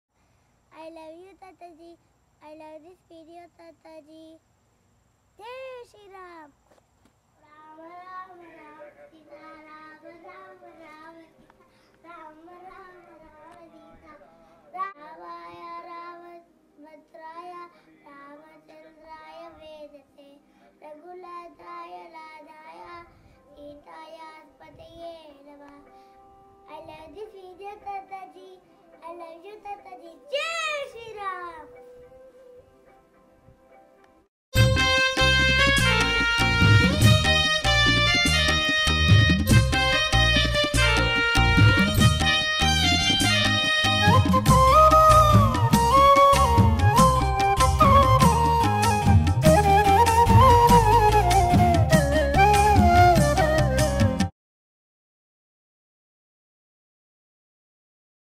Chidatma as Hanuman...Jai Sri Rama..Jai Tataji
If you haven't watched, please watch Shatashloki Ramayana at the above link. In this video 5 year old Chidatma Datta Chaganti poses as Hanuman to express his love towards appaji and the Shata Shloki Video...